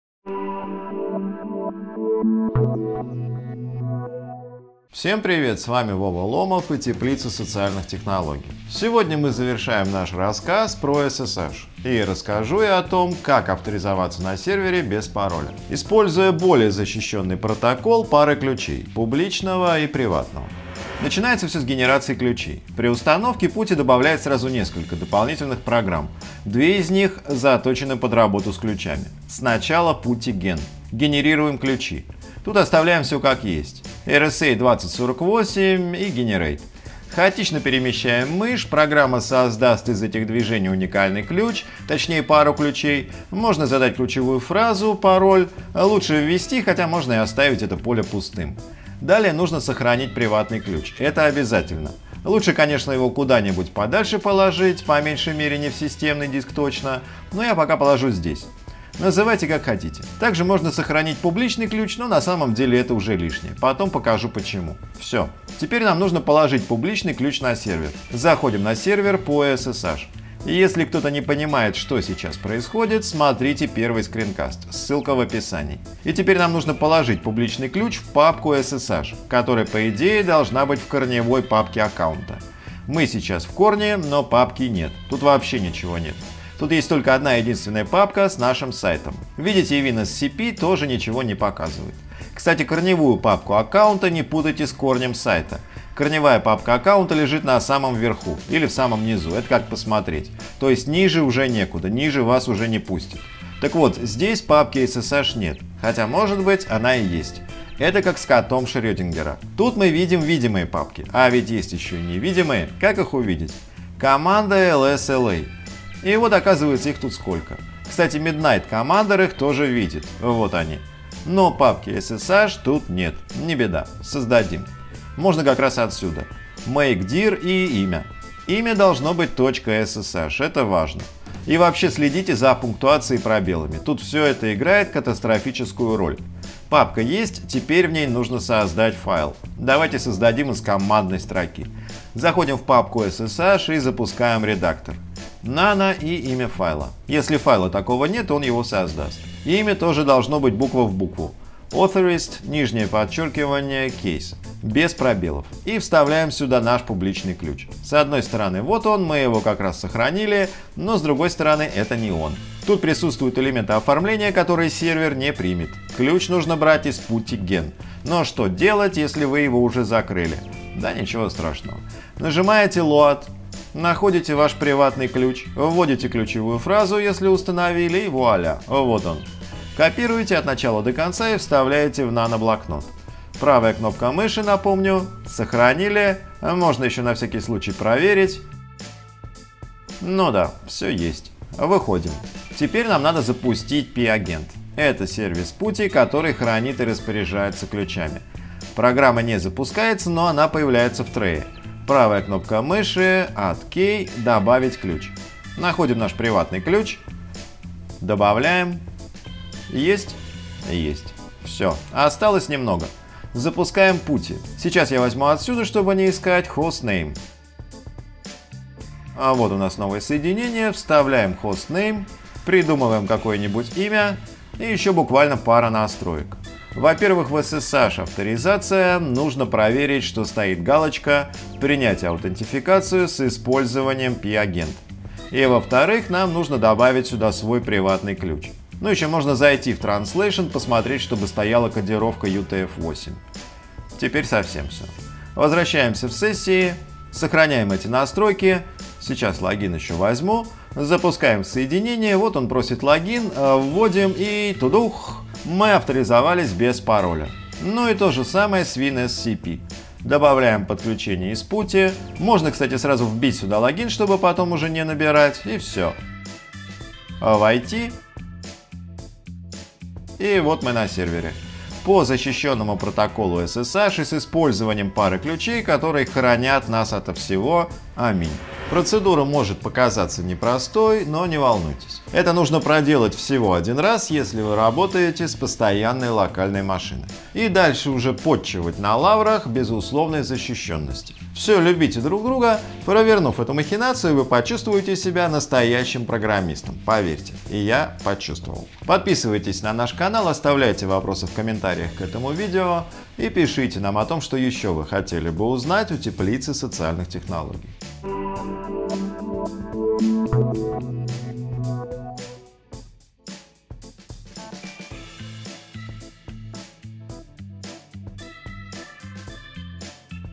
Авторизация по SSH с использованием пары ключей: публичного и приватного
Продолжаем тему работы с сайтом по SSH. Сегодня расскажем о том, как авторизоваться по SSH с использованием пары ключей: публичного и приватного. Это более продвинутая функция по сравнению с авторизацией по паролю, и она удобнее, поскольку сделать это нужно всего один раз и больше вводить пароли не придется. Как, впрочем, не придется и волноваться о безопасности вашего сайта.

SSH (Secure Shell) — это туннель, который обеспечивает безопасную передачу данных между клиентом и сервером. Данные при передаче шифруются, что значительно снижает риск их перехвата и потери. Именно поэтому SSH предпочтительнее незащищенного протокола FTP, а настроить доступ по SSH не многим сложнее, чем по FTP.